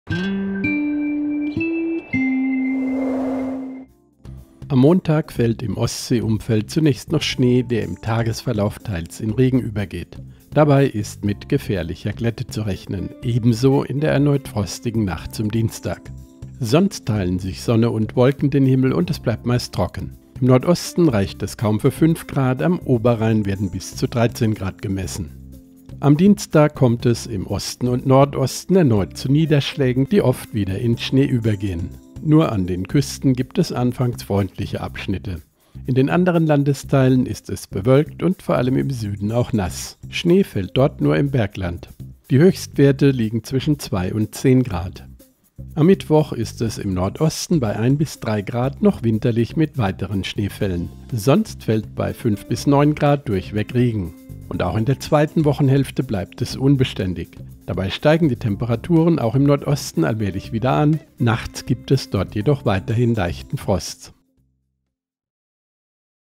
Woche startet im Süden sehr mild (04.03.2018)
Während die neue Woche im Süden und in der Landesmitte recht mild startet, kann sich im Nordosten noch länger kalte Spätwinterluft behaupten. Dort besteht am Montag Glättegefahr durch Schnee oder gefrierenden Regen.